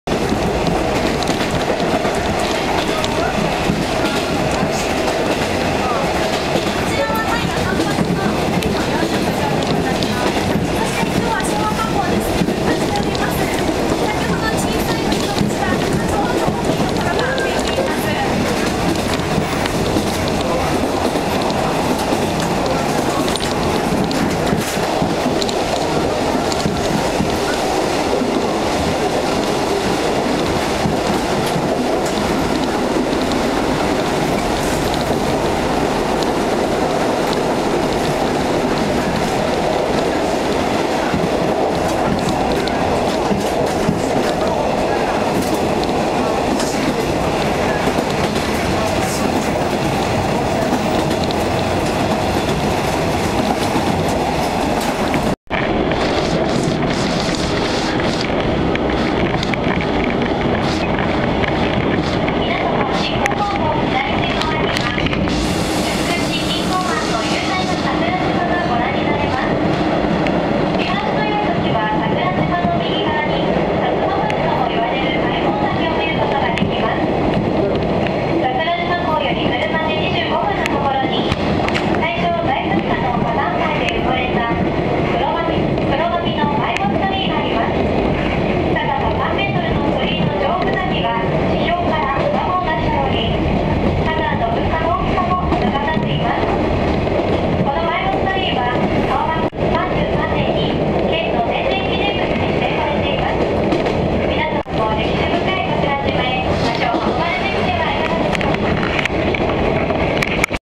JR九州日豊本線・鹿児島本線-はやとの風
観光列車。
鹿児島中央駅-吉松駅の間で運航されている特急。
錦江湾（鹿児島湾）の風景。
車窓。